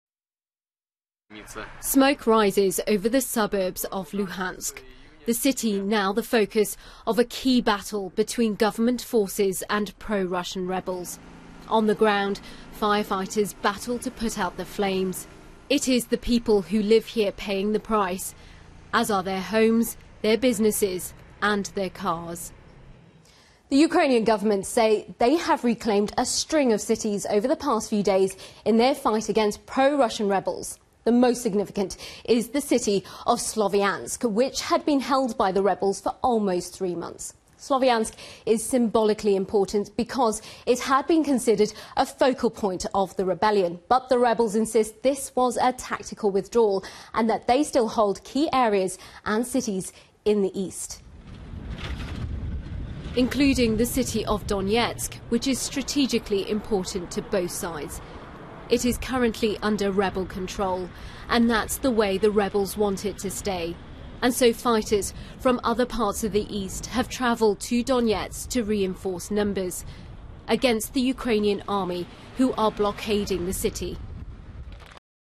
Ukraine captures two more cities from separatist rebels
Ukraine's government says it has retaken two more eastern cities from pro-Russian rebels.

The website of Ukrainian President Petro Poroshenko said the national flag was raised in Artemivsk and Druzhkivka.

It comes after government forces regained control of the rebel stronghold of Sloviansk.